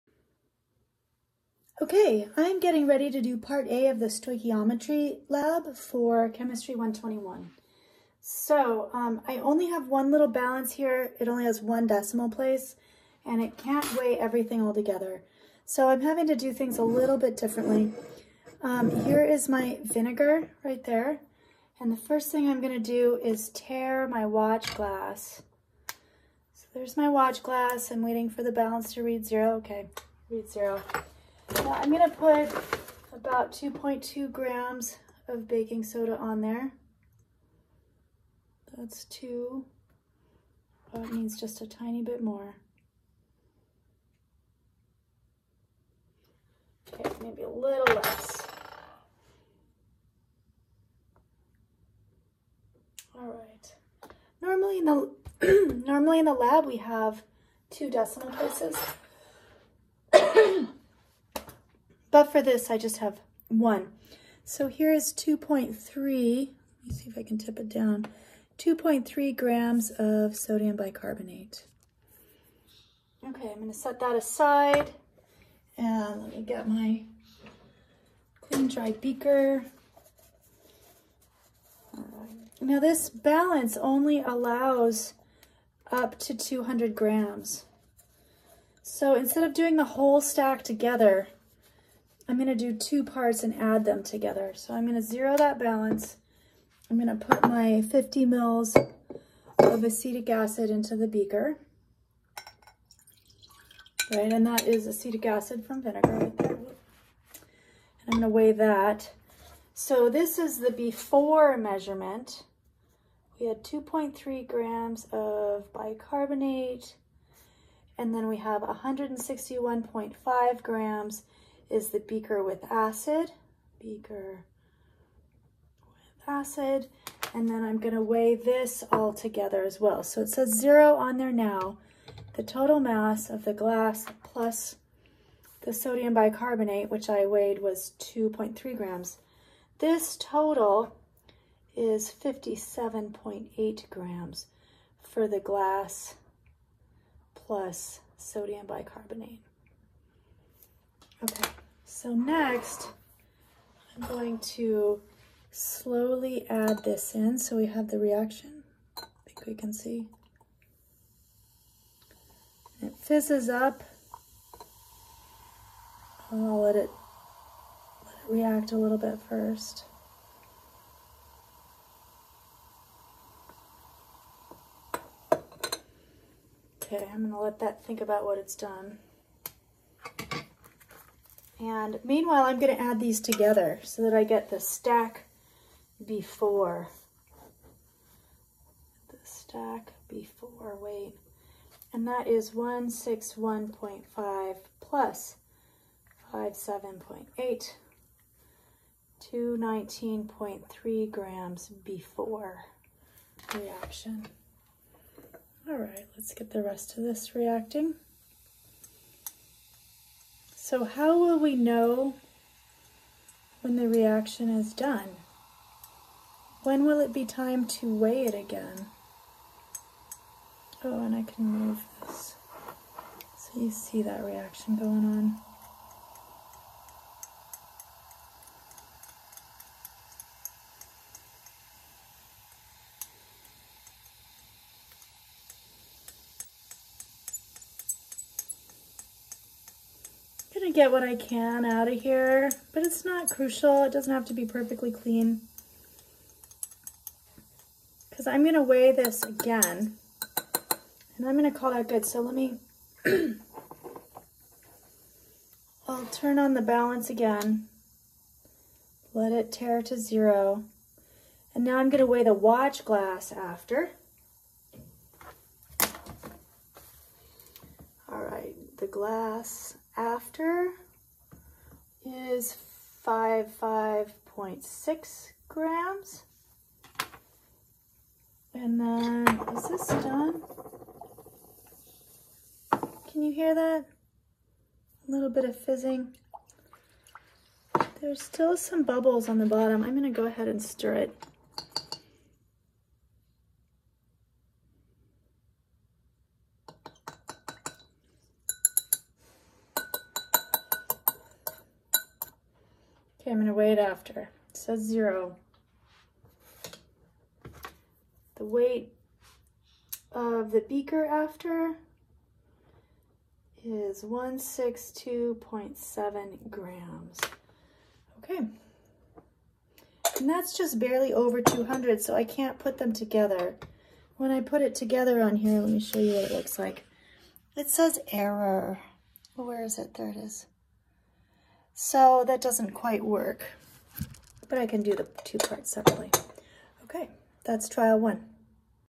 121 Stoichiometry Part A Trial 1
Determining the amount of CO2 released from sodium bicarbonate with excess vinegar.